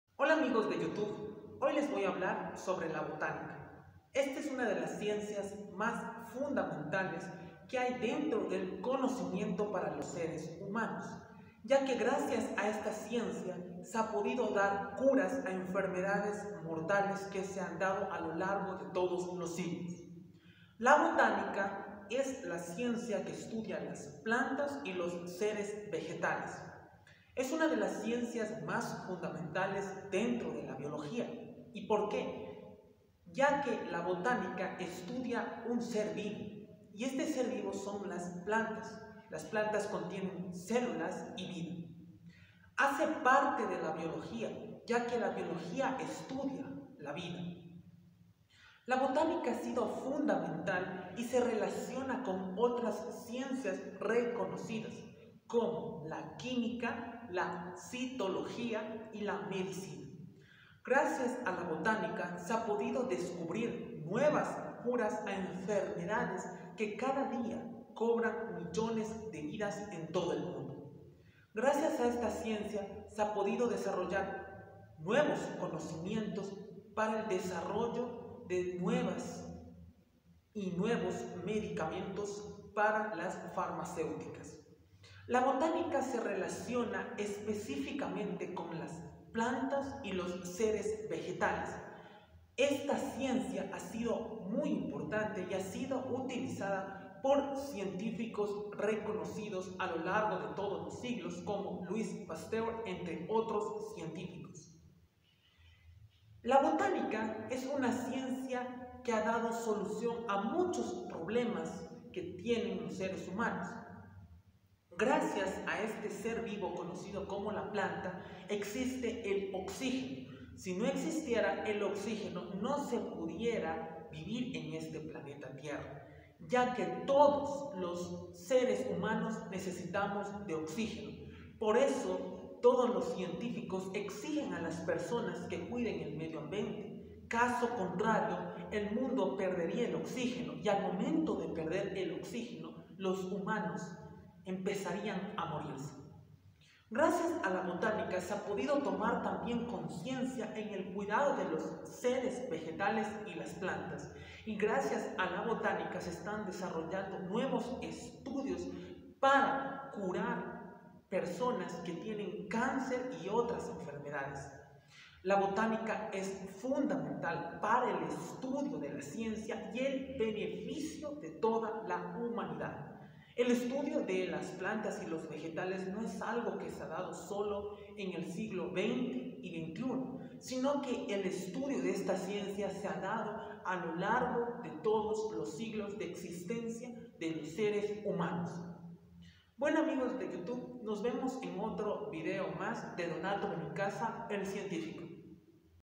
La Botánica
La botánica es una de las ciencias más importantes que hay, ya que gracias a esta se han podido dar curas a enfermedades mortales, es por esto que es importante saber cuales son los beneficios de esta ciencia en las personas.